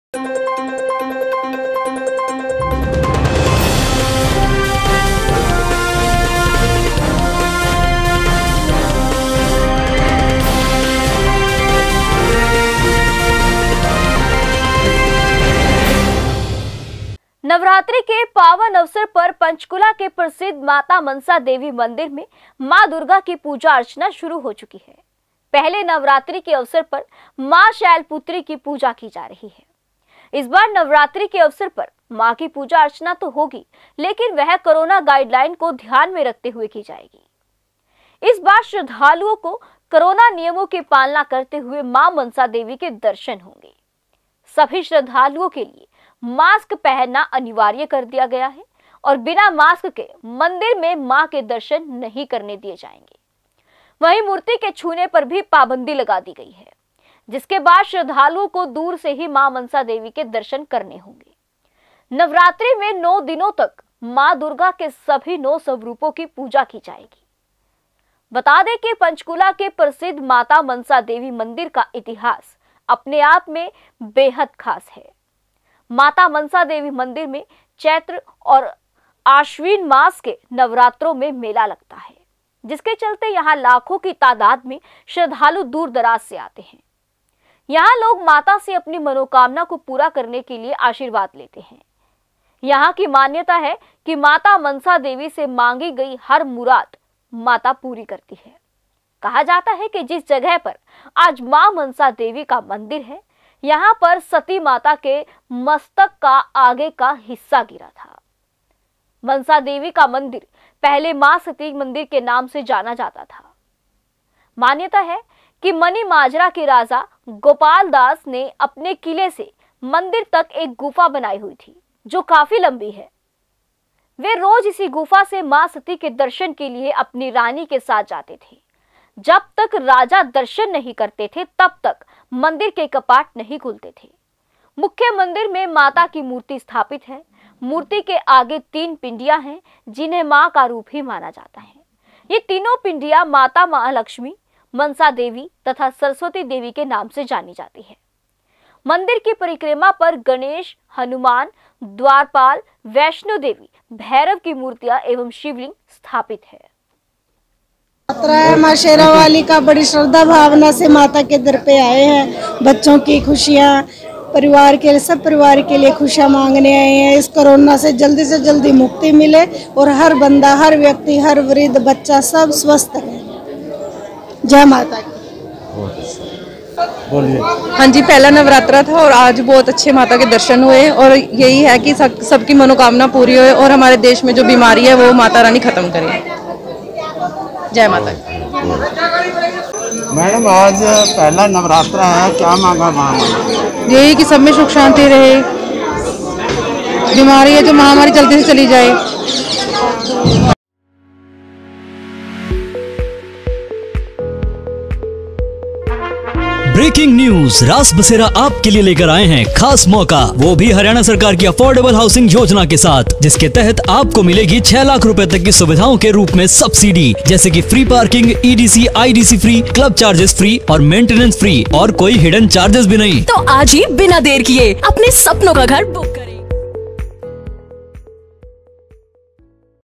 नवरात्रि शुरू, माता मनसा देवी मंदिर में होगी मां दुर्गा की पूजा अर्चना
नवरात्रि के पावन अवसर पर पंचकूला के प्रसिद्ध माता मनसा देवी मंदिर में मां दुर्गा की पूजा-अर्चना शुरू हो चुकी है। पहले नवरात्रि के अवसर पर मां शैलपुत्री की पूजा की जा रही है। इस बार नवरात्रि के अवसर पर मां की पूजा-अर्चना तो होगी लेकिन वह कोरोना गाइडलाइन्स को ध्यान में रखते हुए की  जाएगी। इस बार श्रद्धालुओं को कोरोना नियमों की पालना करते हुए मां मनसा देवी के दर्शन होंगे। सभी श्रद्धालुओं के लिए मास्क पहनना अनिवार्य कर दिया गया है और बिना मास्क के मंदिर में मां के दर्शन नहीं करने दिए जाएंगे।